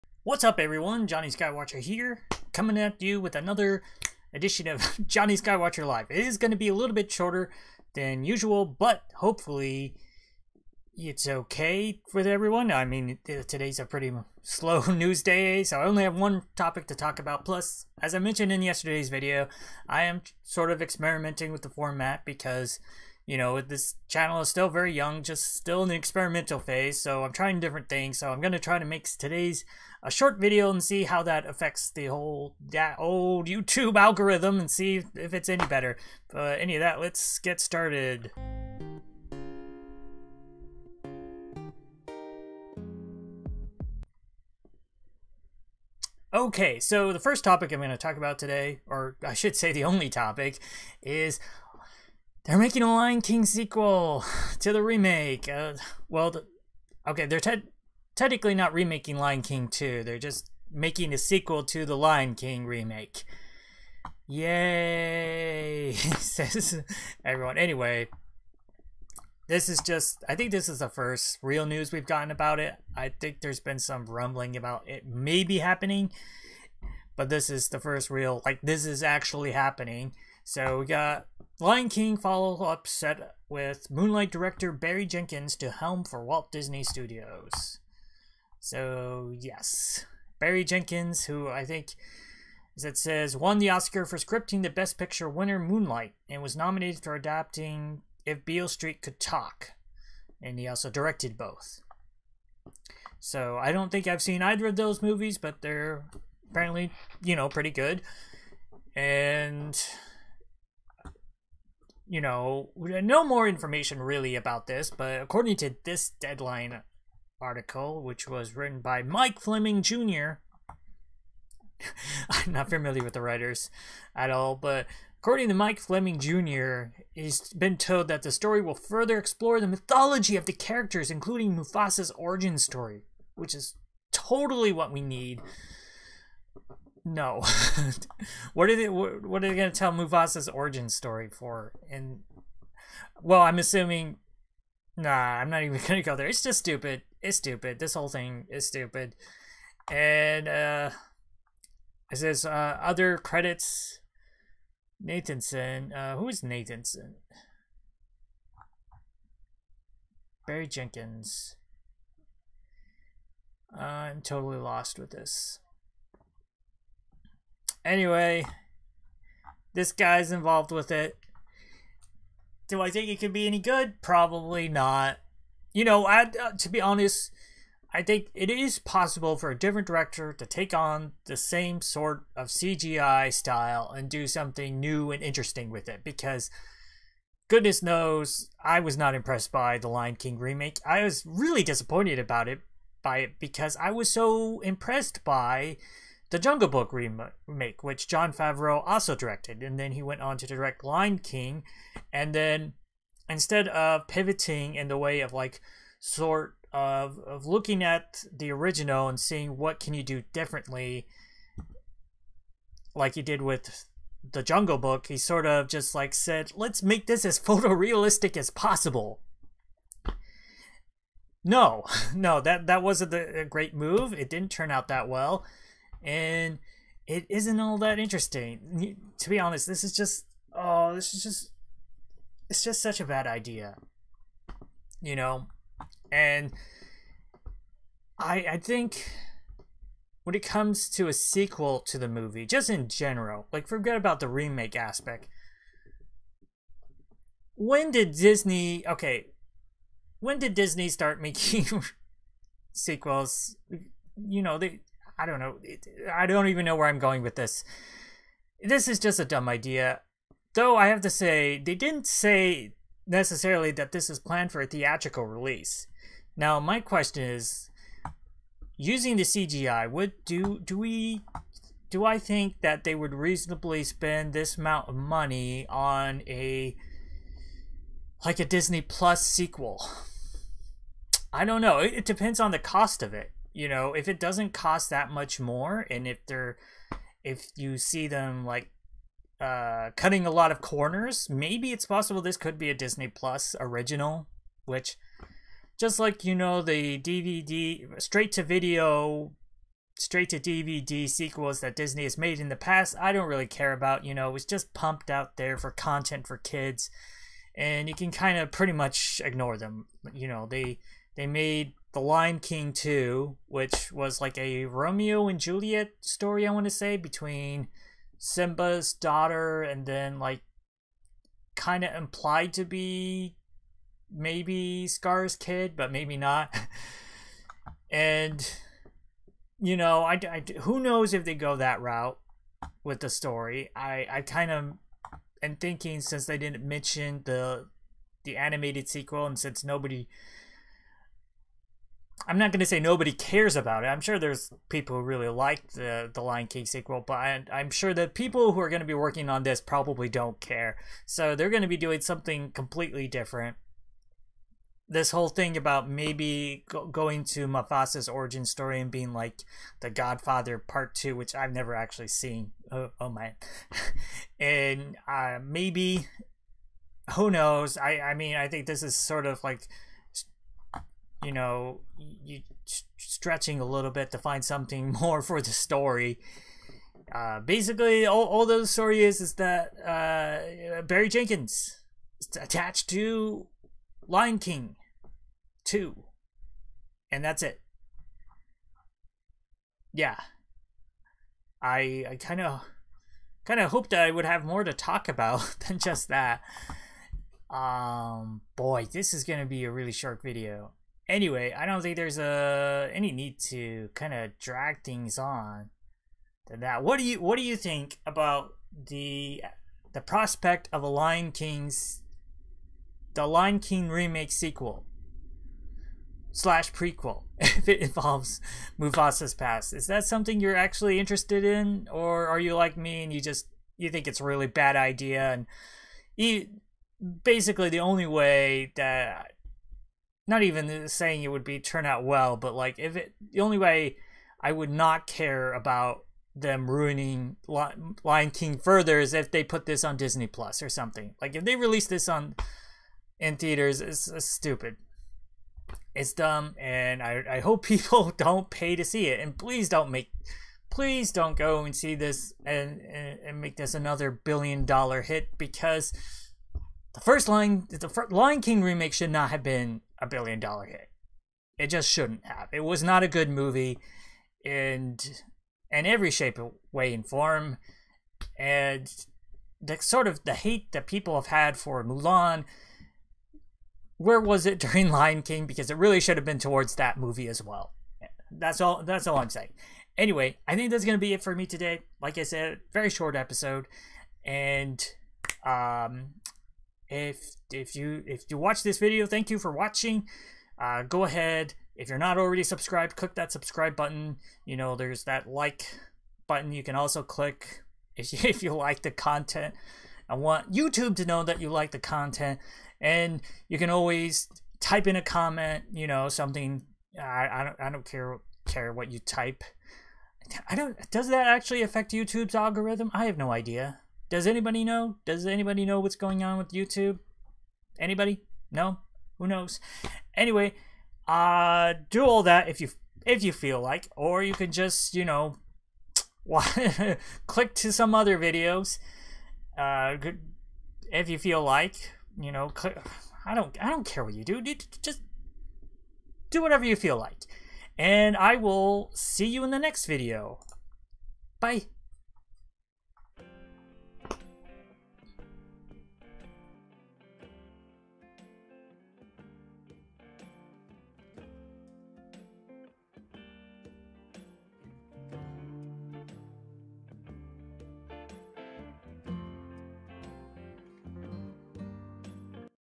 Lion King 2: Electric Boogaloo
Jonny Skywatcher Live

Disney wants to make another Lion King because the first one made money and that is all that matters to a corporation like Disney.